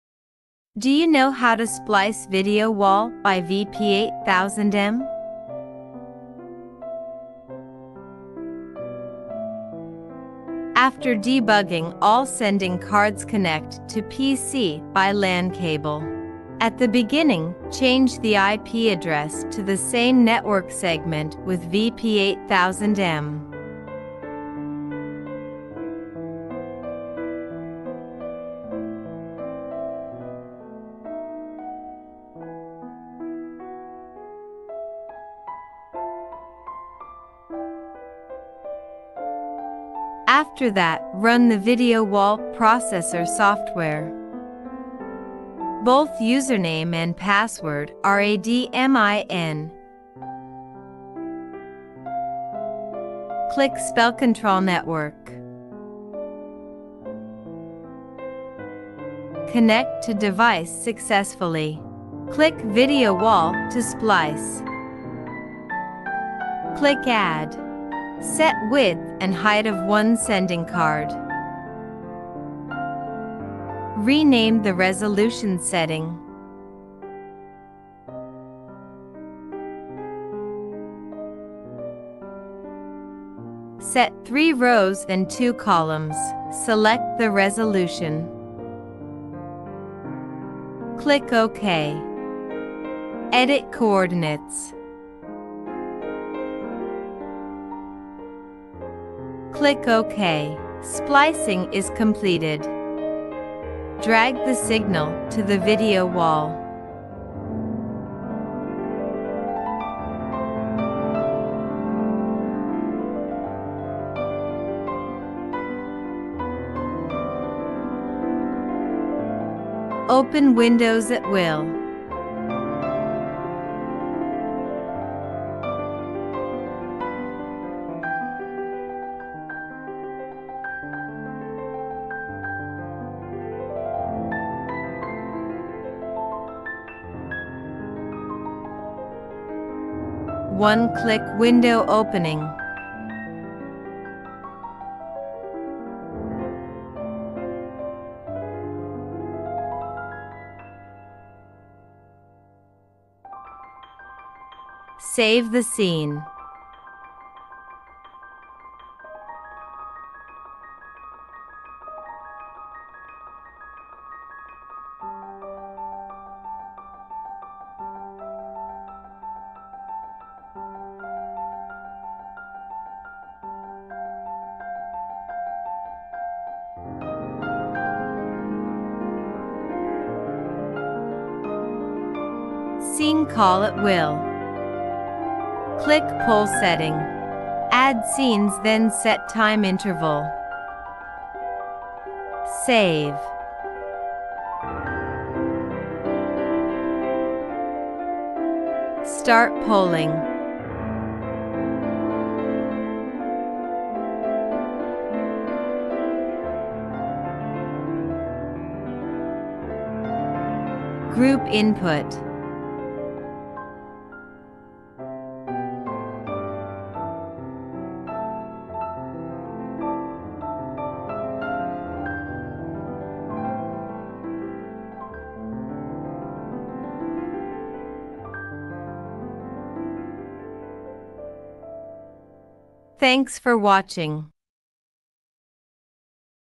Huidu Technology LED Wall Splicer Controller VP8000M Video tutoials
vp8000M LED Controller integtated with  image splicing processor , iti is a hybrid card-type intelligent image splicing processor, which can display multiple dynamic images on multiple
On the screen, the function of multi-window splicing is realized. This product can automatically detect and identify the board type, support DVI, HDMI
And other splicing boards, plug and play, and support video splicing, control third-party equipment and other functions.
Device controlled methods include: RS232 control and LAN client software control, and supports serial port control of remote third-party devices
prepare. Using the client control software, you can set a variety of output screen splicing display, input signal OSD settings,
Scene saving, scene polling, etc.
The image stitching processor can be widely used in urban security monitoring, intelligent traffic management, video conferencing, large conference centers,
Large commercial squares, military command centers, government agencies and other occasions.
Professional LED Screen controller manufacturer